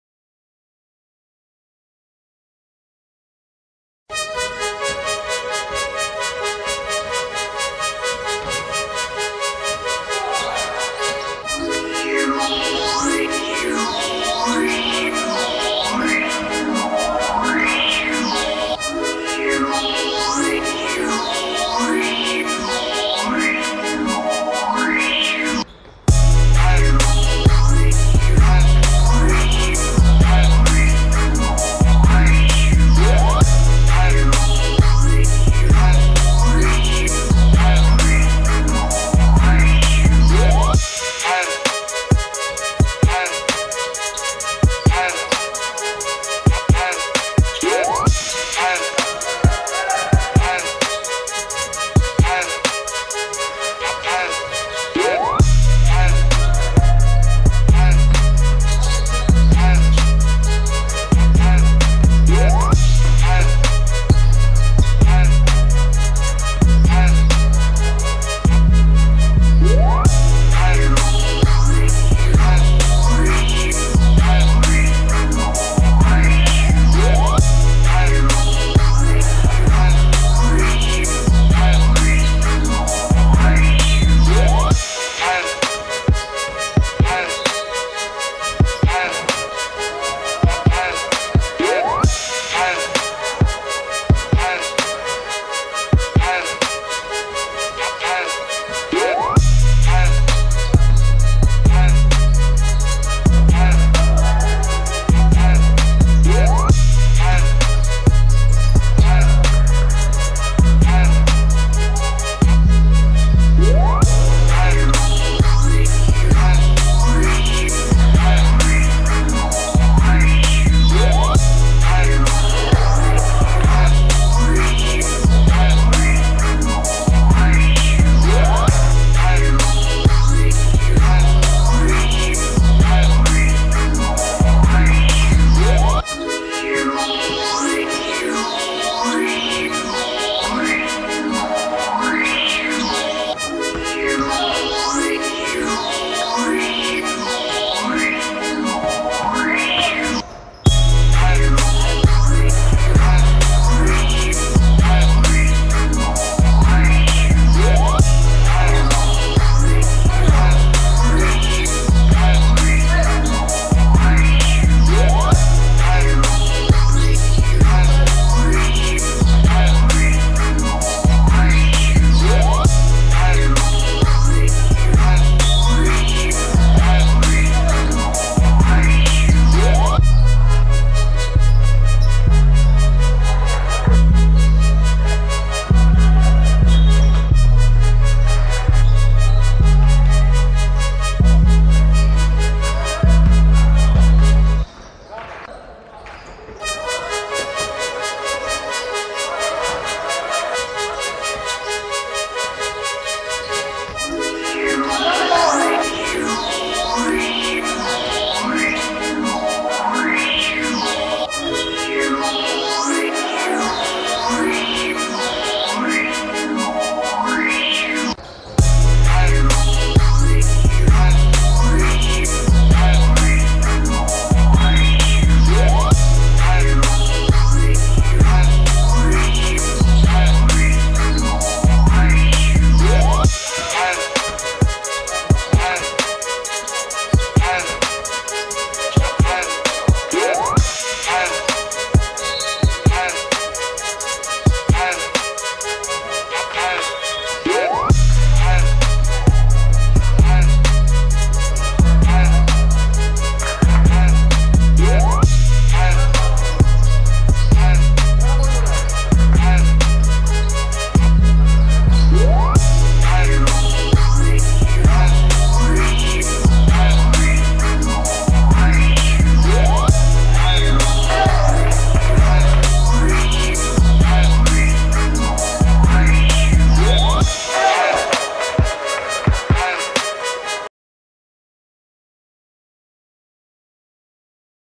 18 03 Tigers vs Gators – Prospect Div Zima 2022 23 r
Tigers 49:77 Gators
J.Żuławski 11pkt 9zb 8ast 7stl
M.Konieczny 25pkt 5zb